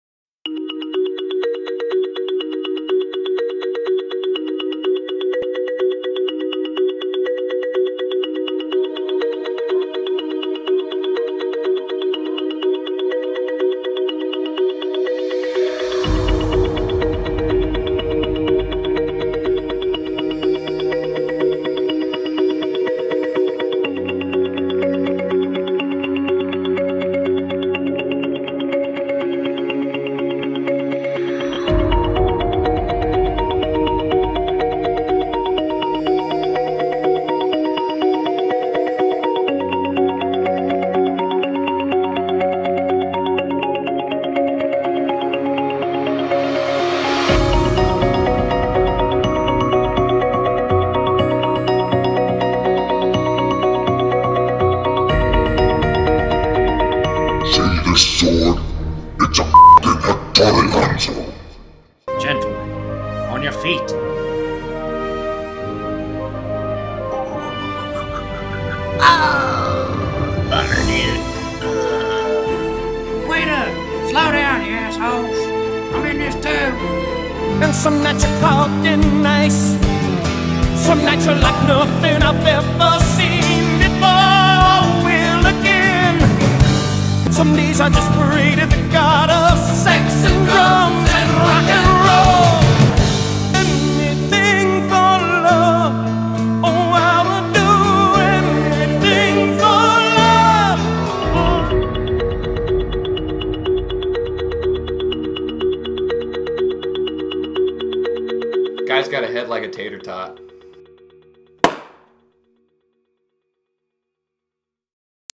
K. Akers Animated Demo Reel
K.Akers' 2D Animation Skills: Composite photography and imagery combined with motion graphic effects. Animated using Photoshop and Final Cut Studio. Some combination live-action footage. Influences to the style are Monty Python, South Park, Robot Chicken, Quasi Stop-Motion, cut-outs